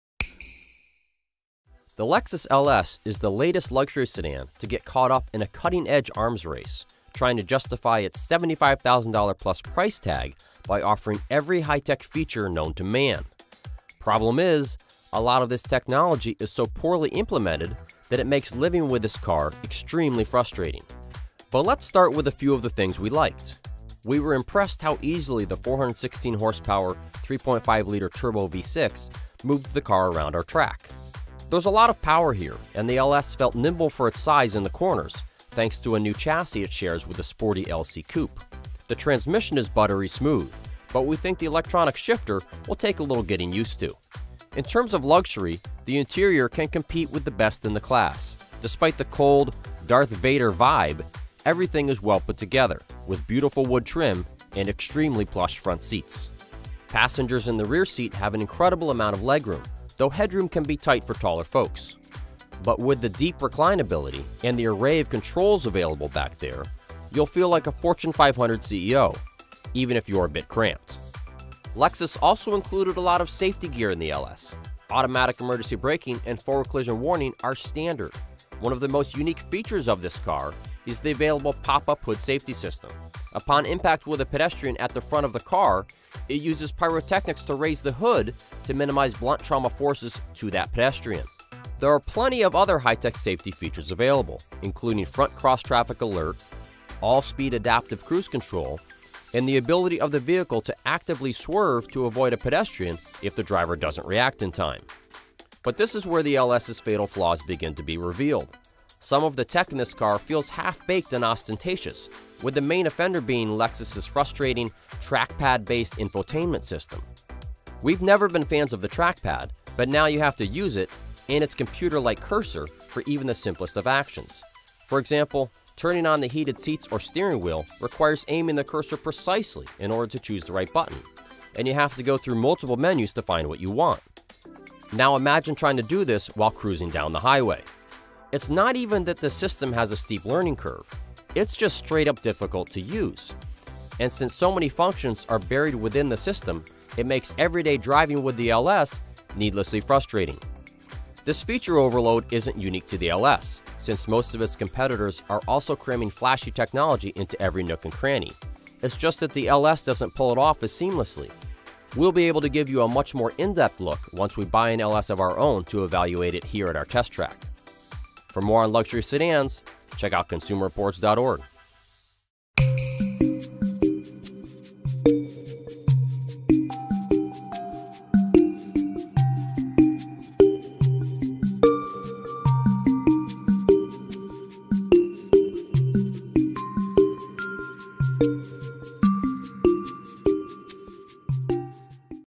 2018 Lexus LS Quick Drive | Consumer Reports
The redesigned 2018 Lexus LS concentrates heavily on technology. But much of that technology is poorly implemented and can be extremely frustrating, including the trackpad-based infotainment system.